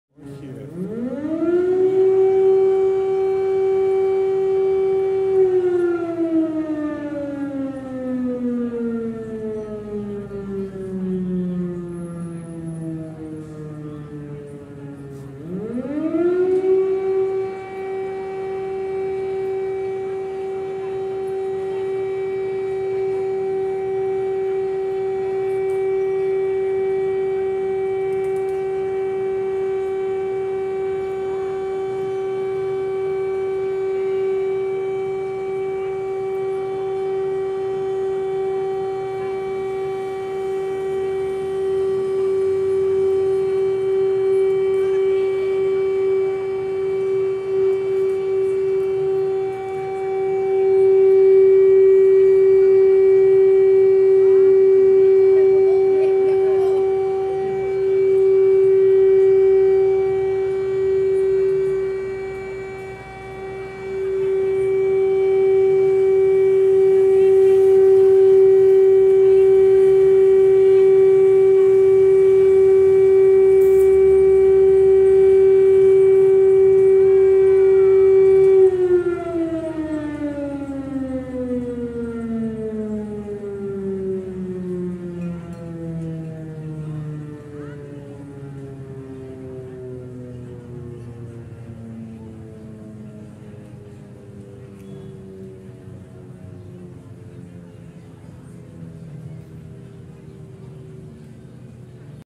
Καβάλα:Ήχησαν, δοκιμαστικά, οι σειρήνες
Ήχησαν δοκιμαστικά σε όλη τη χώρα και στην Καβάλα,οι σειρήνες του πολέμου, στα πλαίσια της ασκησης Παρμενων